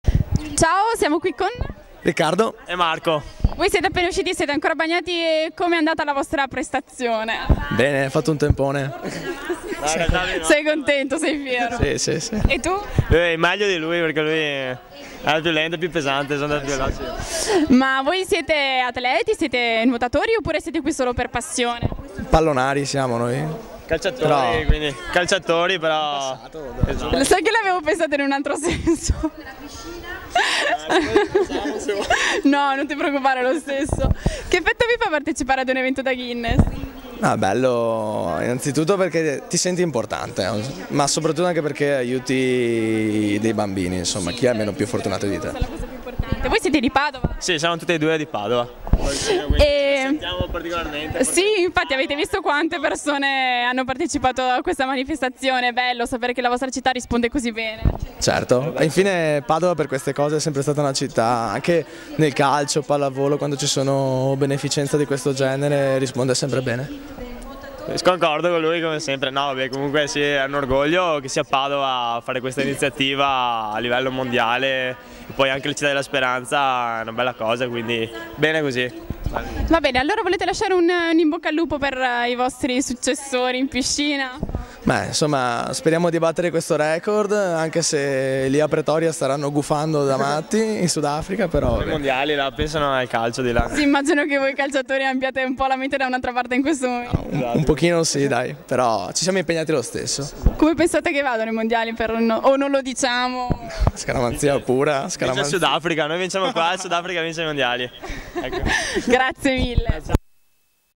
Bauli a Swim For Children - Intervista ai calciatori Davide e Marco!
Le interviste realizzate durante la steffetta Swim For Children. Le emozioni da Guinnes raccontate alla telecamera di dolce far nuoto!